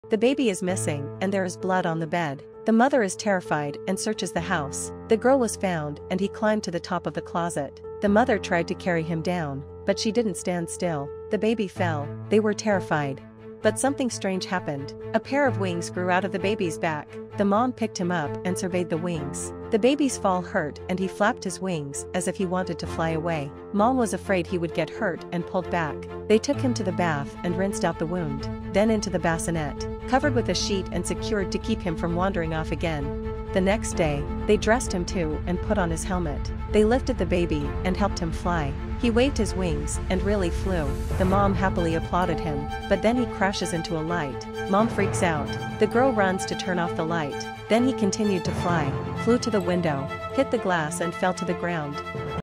The baby in the bassinet is gone.
The mother is terrified and searches the house. The girl was found and he climbed to the top of the closet. The mother tried to carry him down.